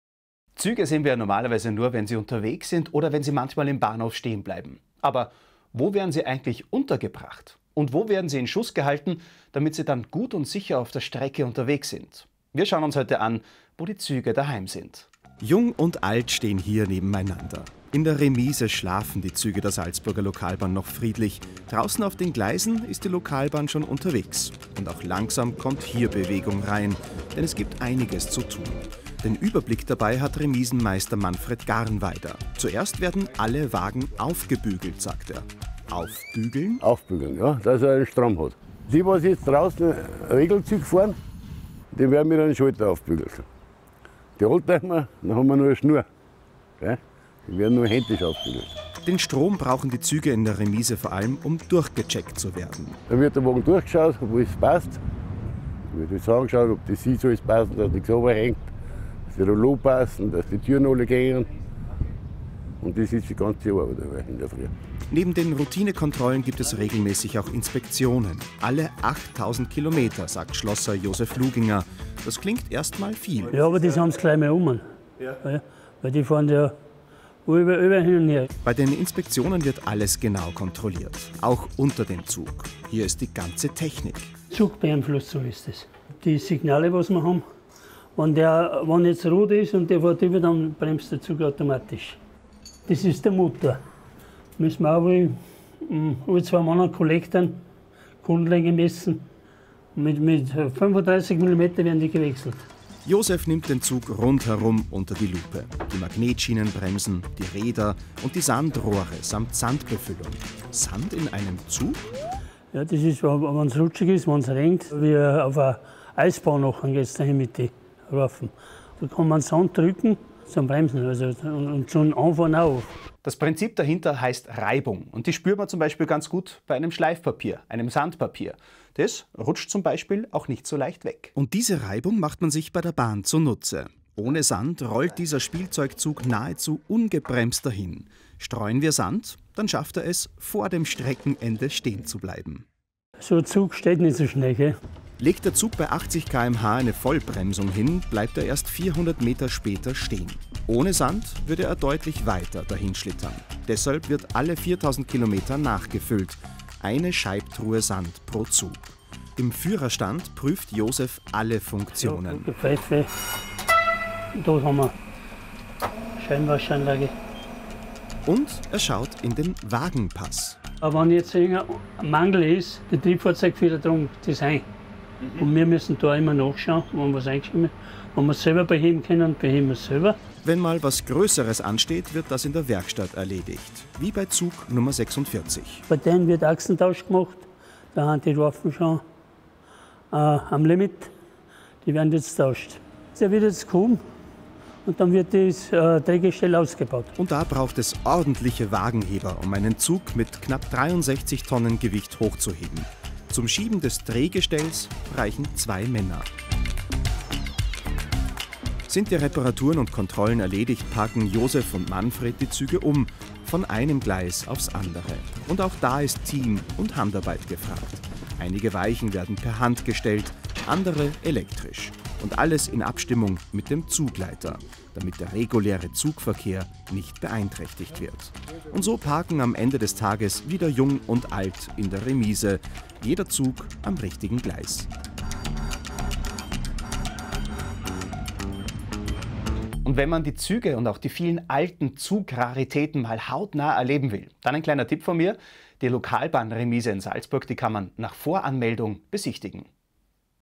Wissen kompakt - Lokalbahnremise🚂
Wo werden Züge untergebracht? Das GreenTech Unternehmen Salzburg AG präsentiert eine kompakte Erklärung dieser Lokalbahnremise. Hier wird von der Inspektion, über Sand auf den Gleisen bis hin zur Reparatur alles thematisiert.

00:00 Begrüßung
00:16 "aufbügeln"
01:09 Inspektionen
01:48 Sand im Zug
03:02 Achsentausch
03:31 parken
04:00 Abschluss